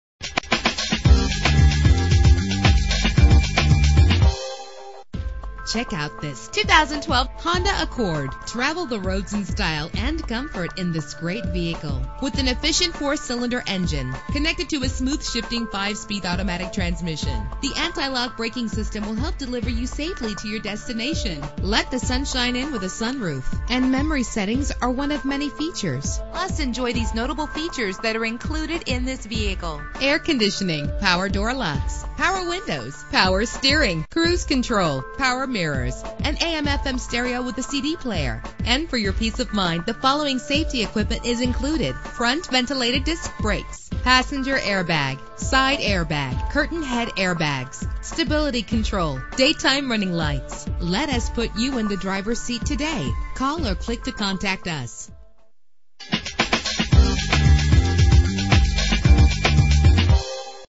2012 Honda Accord - Old Bridge NJ
Year: 2012
Make: Honda
Model: Accord
Trim: EX-L
Engine: 2.4 liter inline 4 cylinder 16 valve
Transmission: 5-Speed Automatic
Color: Basque Red
Mileage: 10
Address:
1101 US highway 9